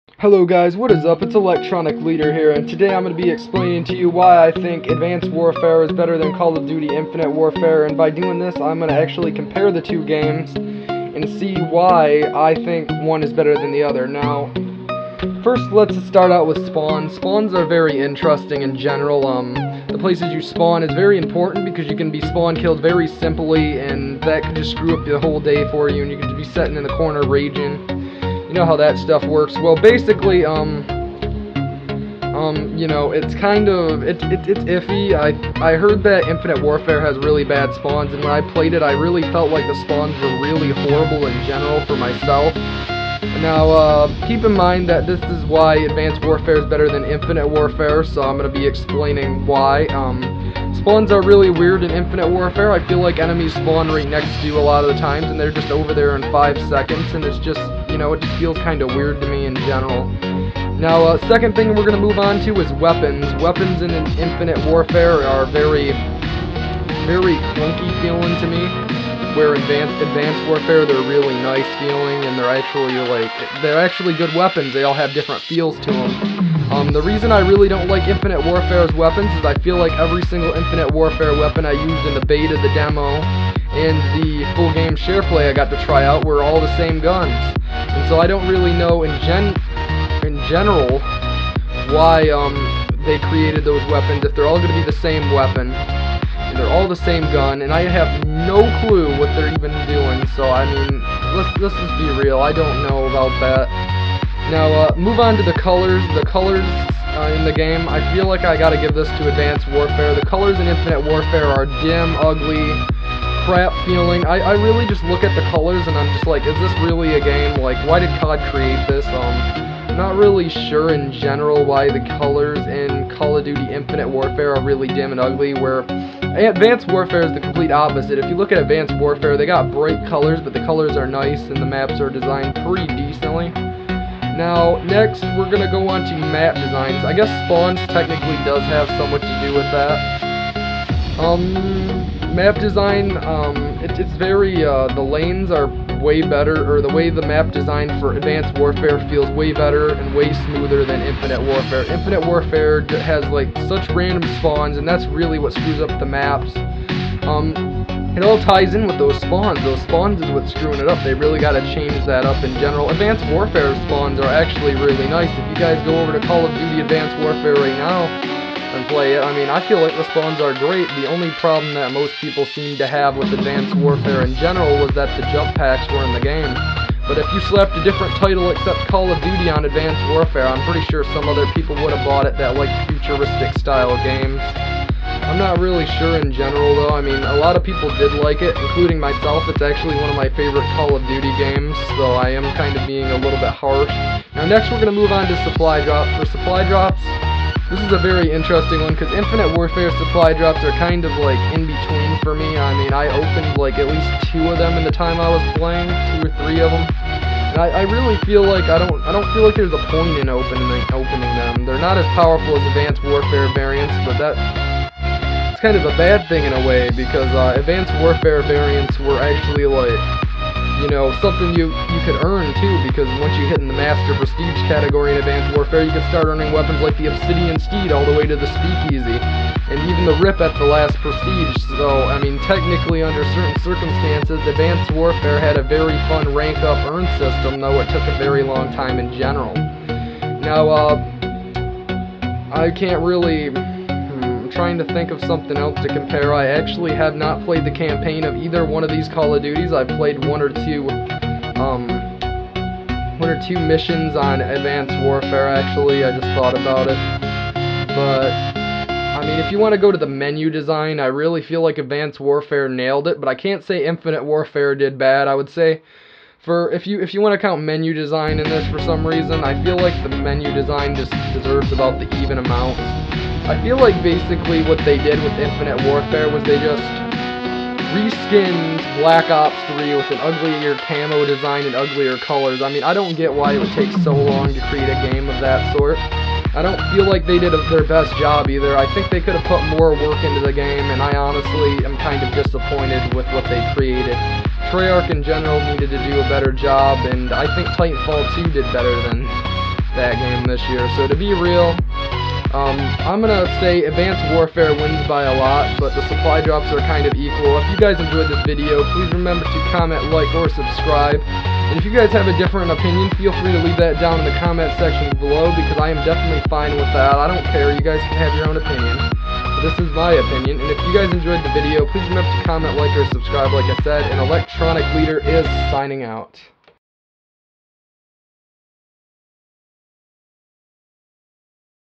Why i like cod aw more than infinite warfare
If you got a opinion leave it down in the comment section below and remember this is just my opinion.